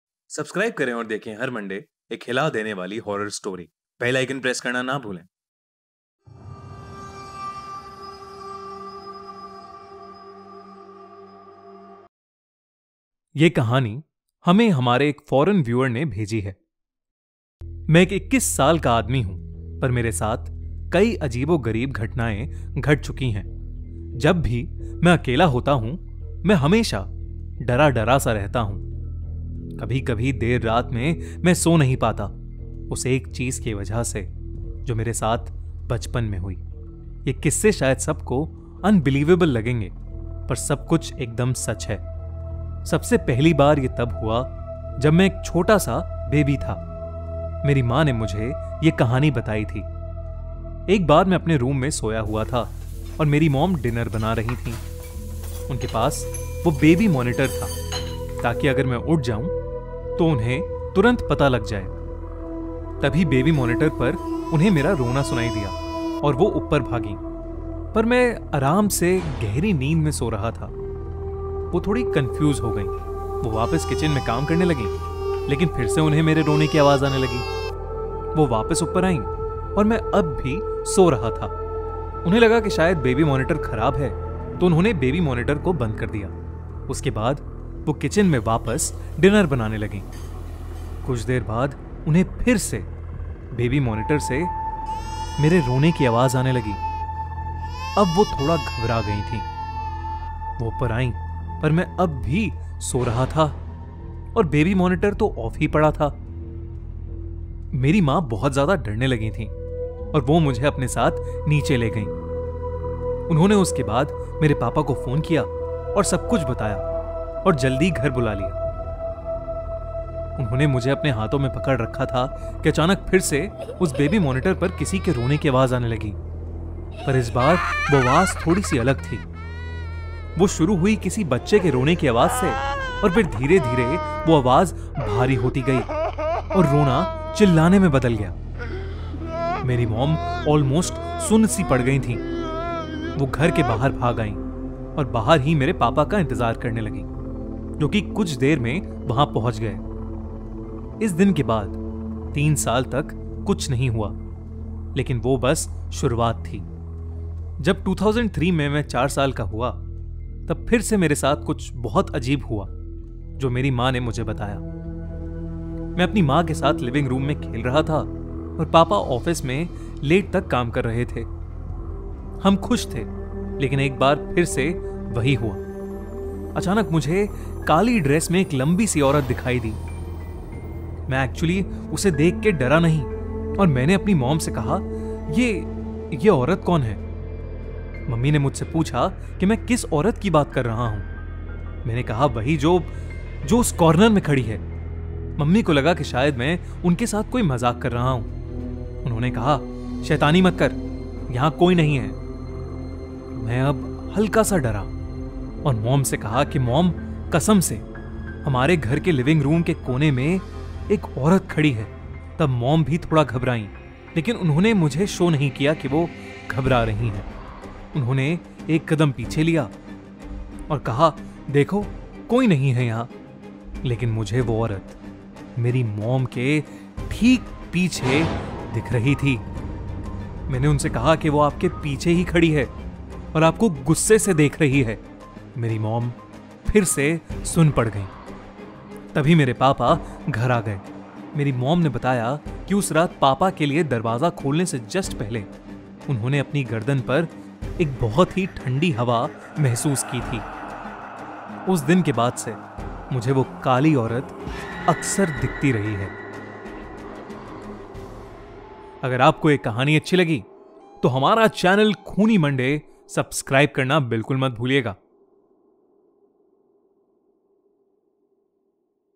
Crying Baby Horror Story In Hindi | सच्ची कहानी | KM E108 🔥🔥🔥
This story happened with me when I was just a baby. That day, I cried and cried.